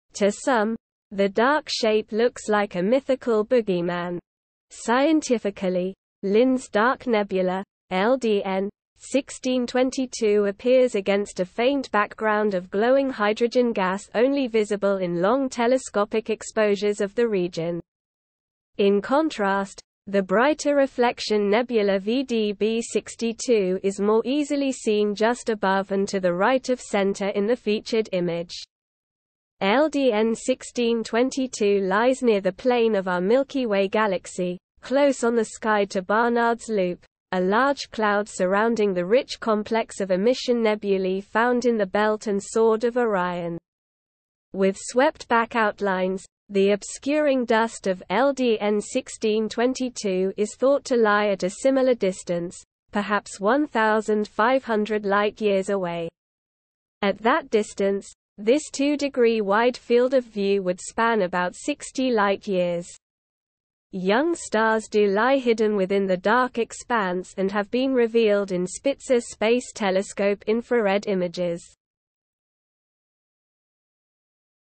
LDN 1622: The Boogeyman Nebula
To some, the dark shape looks like a mythical boogeyman.  Scientifically, Lynds' Dark Nebula (LDN) 1622 appears against a faint background of glowing hydrogen gas only visible in long telescopic exposures of the region. In contrast, the brighter reflection nebula vdB 62 is more easily seen just above and to the right of center in the featured image. LDN 1622 lies near the plane of our Milky Way Galaxy, close on the sky to Barnard's Loop, a large cloud surrounding the rich complex of emission nebulae found in the Belt and Sword of Orion. With swept-back outlines, the obscuring dust of LDN 1622 is thought to lie at a similar distance, perhaps 1,500 light-years away. At that distance, this 2-degree wide field of view would span about 60 light-years. Young stars do lie hidden within the dark expanse and have been revealed in Spitzer Space Telescope infrared images.

Image Credit & Copyright: Joshua Carter